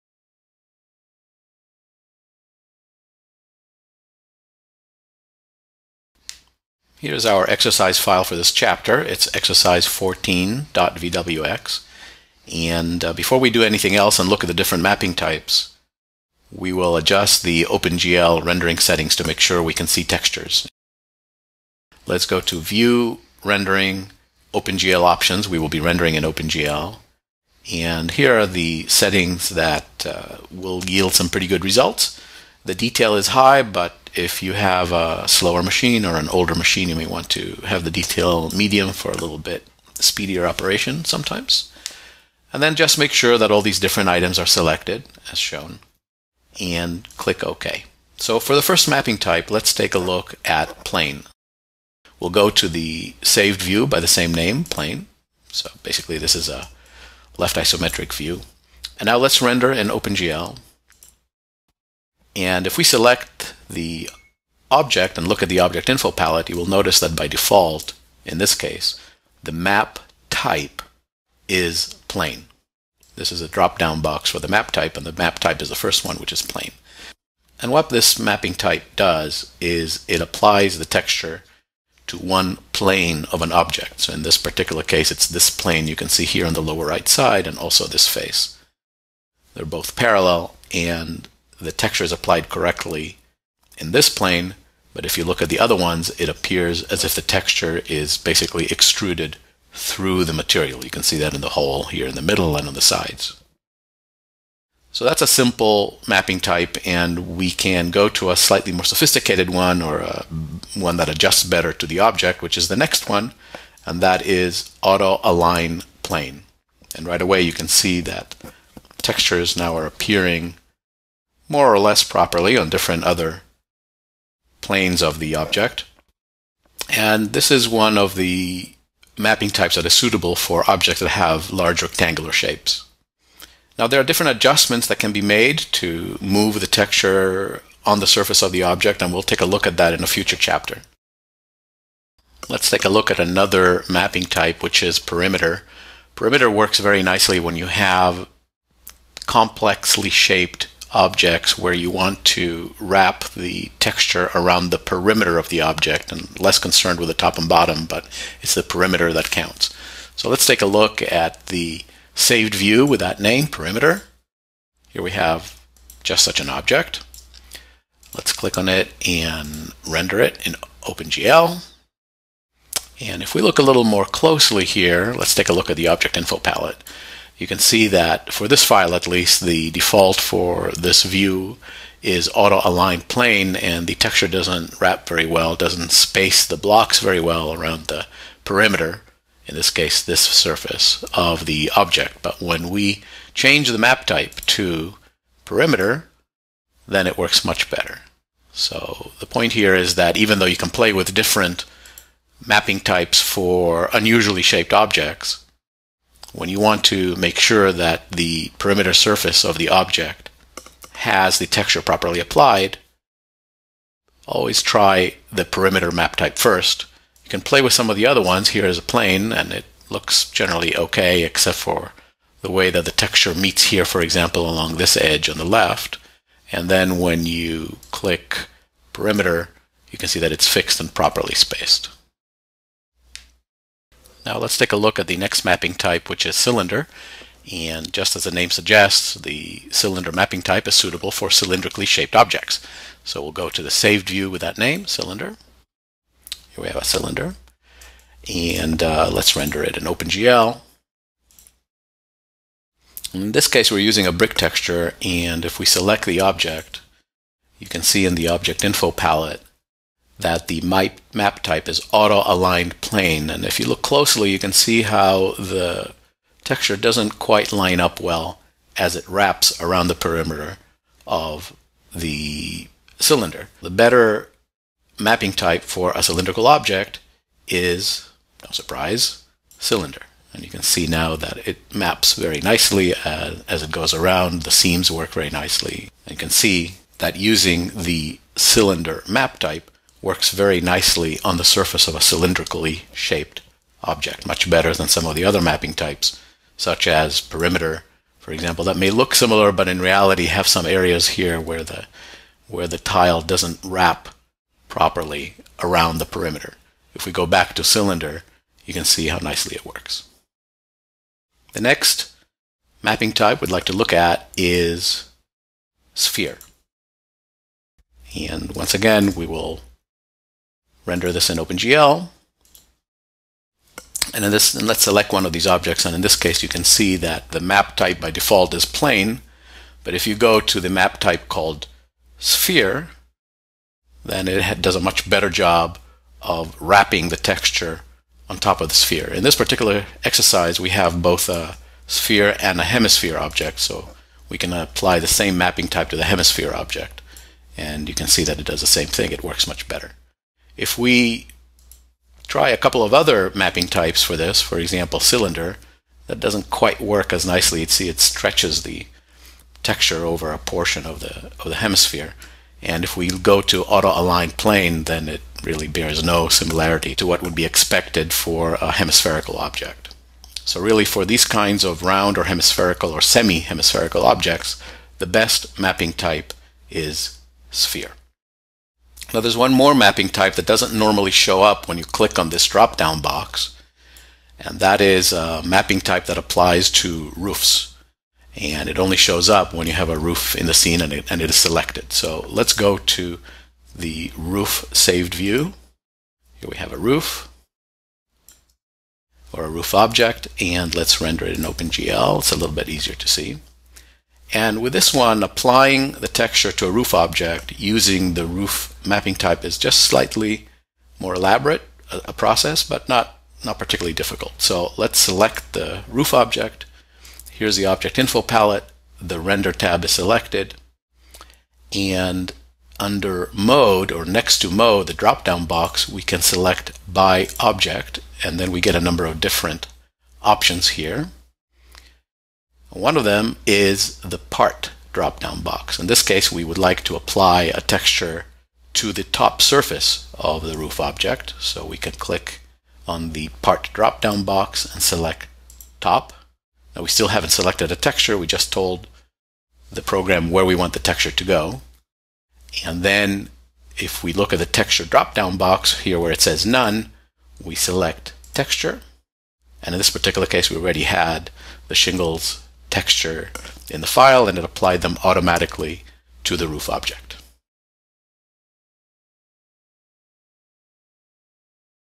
Remarkable Renderworks Ep. 14: Texture Mapping Types
Take a look at how to apply textures on single planes, perimeters, and more with architect and rendering expert Daniel Jansenson, using texture mapping types in Vectorworks software’s built-in Renderworks features.